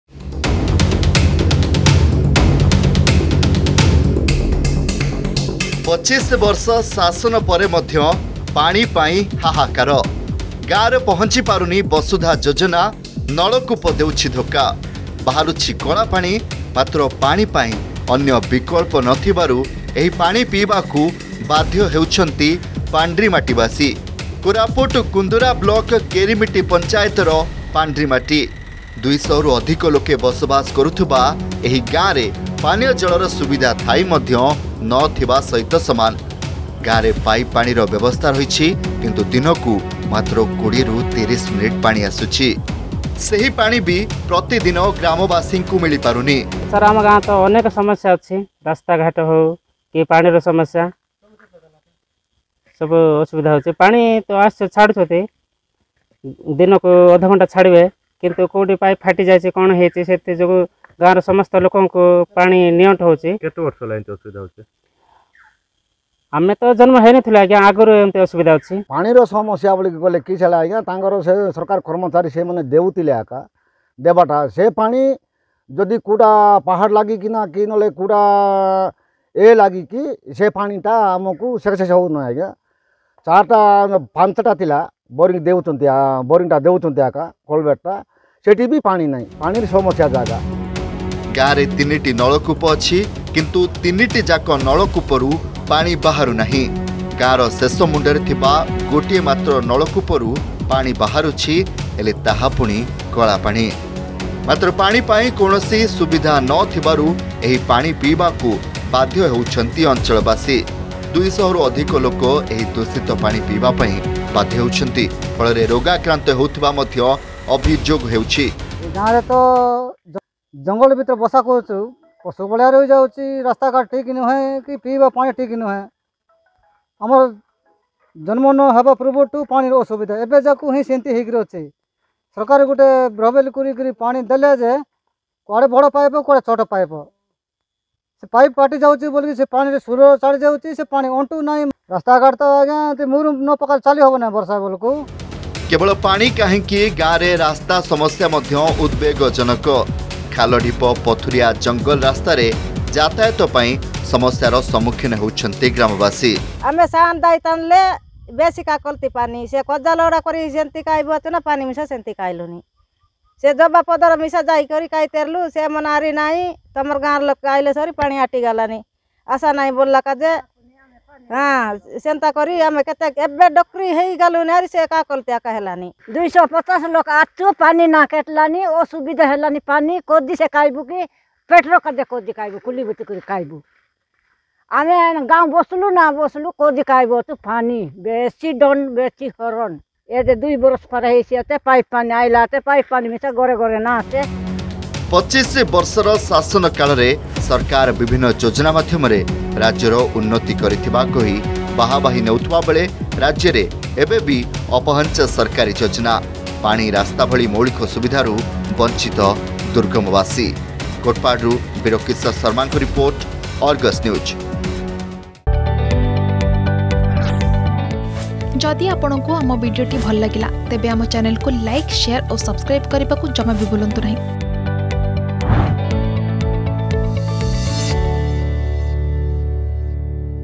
25 ବର୍ଷ ଶାସନ ପରେ ମଧ୍ୟ ପାଣି ପାଇଁ ହାହାକାର || Water Problem
25 ବର୍ଷ ଶାସନ ପରେ ମଧ୍ୟ ପାଣି ପାଇଁ ହାହାକାର । ଗାଁରେ ପହଞ୍ଚି ପାରିନି ବସୁଧା ଯୋଜନା। ନଳକୂପ ଦେଉଛି ଧୋକା। ବାହାରୁଛି କଳା ପାଣି। ମାତ୍ର ପାଣି ପାଇଁ ଅନ୍ୟ ବିକଳ୍ପ ନଥିବାରୁ ଏହି ପାଣି ପିଇବାକୁ ବାଧ୍ୟ ହେଉଛନ୍ତି ପାଣ୍ଡ୍ରିମାଟିବାସୀ।